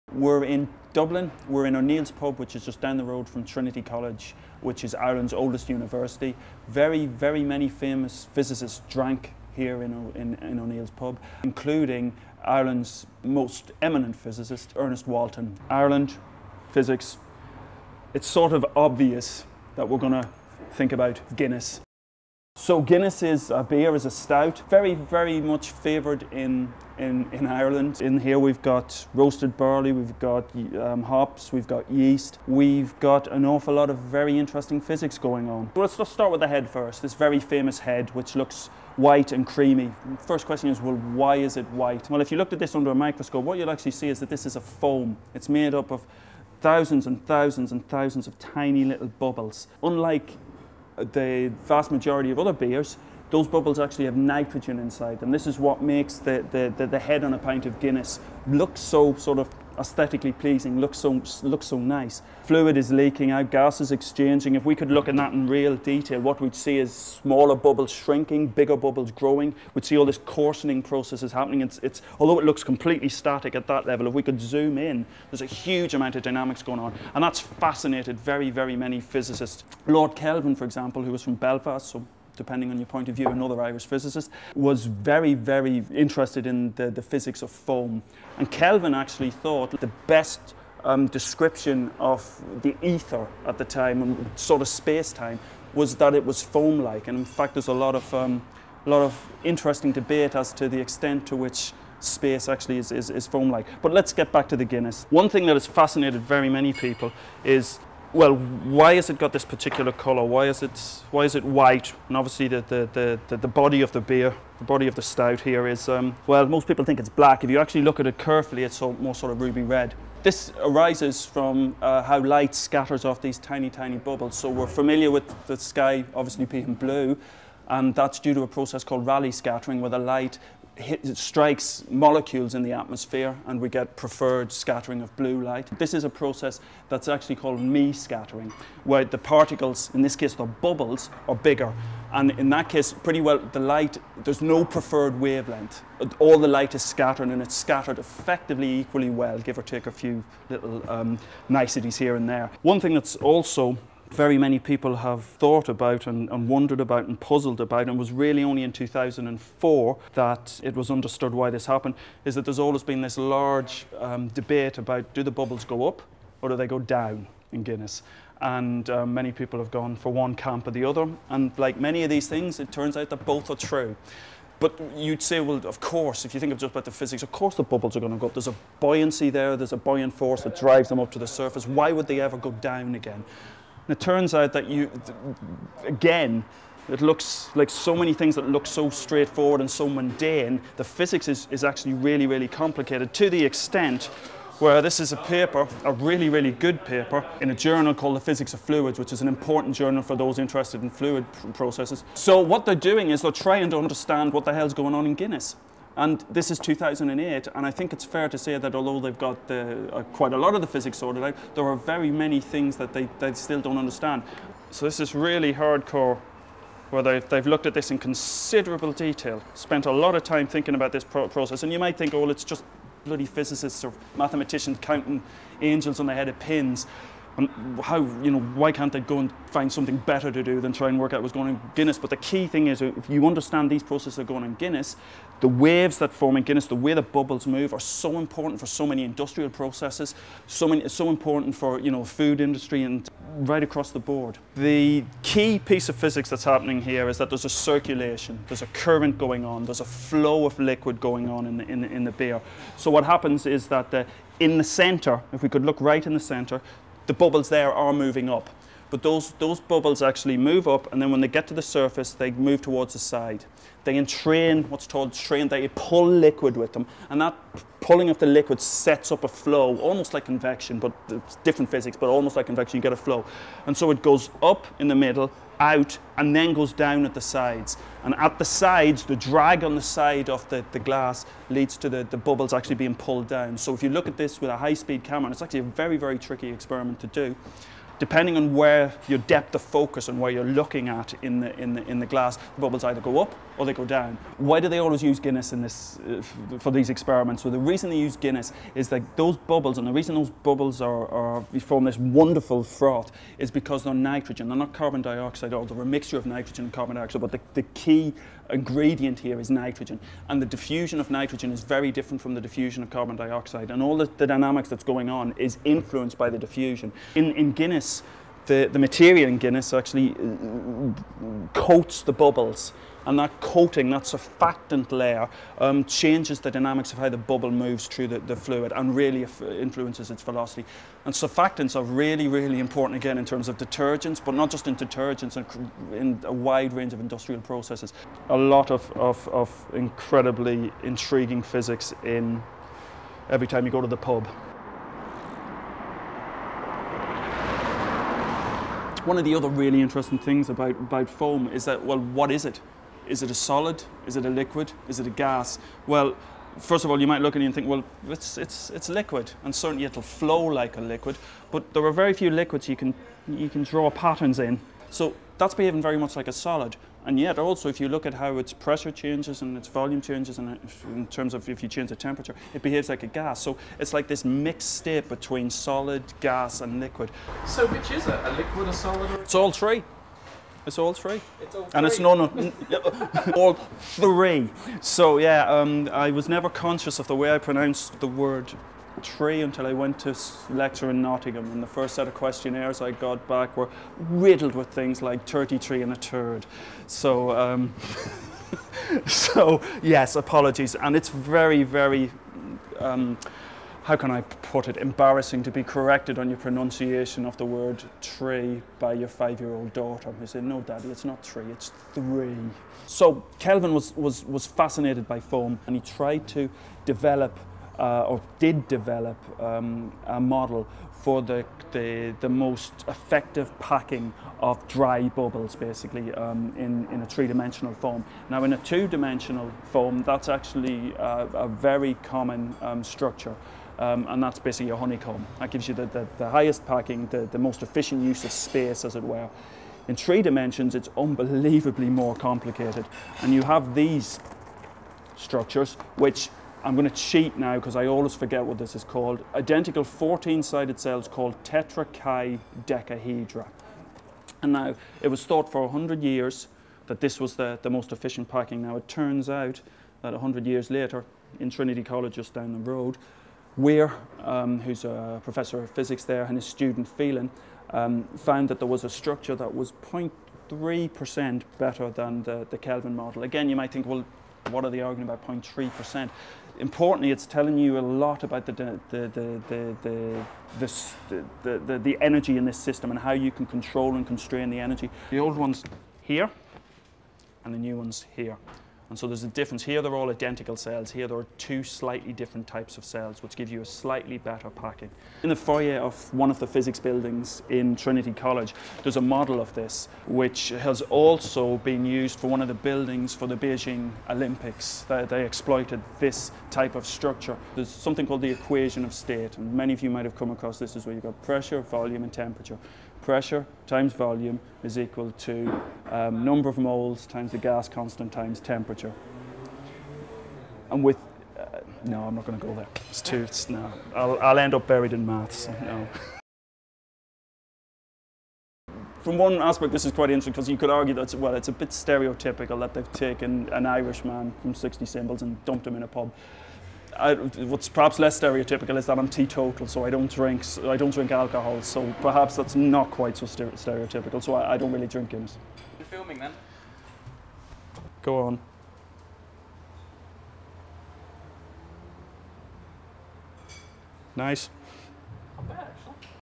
Guinness Science - Sixty Symbols
Strange bubbles and foam are part of the allure of Guinness - at least when you discuss it with an Irish physicist.

Sixty Symbols is in Dublin discussing the nation's much-loved beverage.

With Philip Moriarty